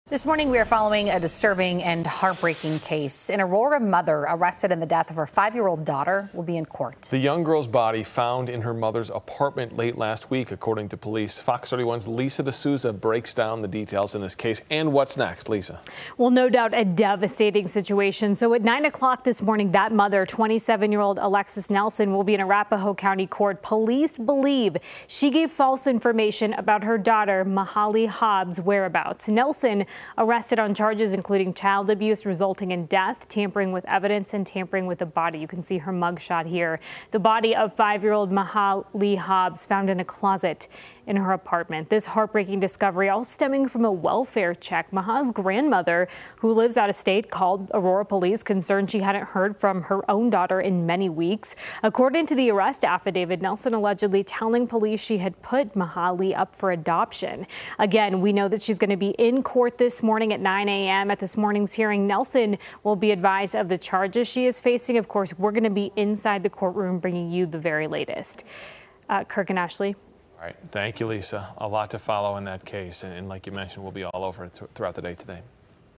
Mother appears in court after daughter found dead
Alexus Nelson will be in court Tuesday on charges of child abuse resulting in death. Lisa D'Souza reports.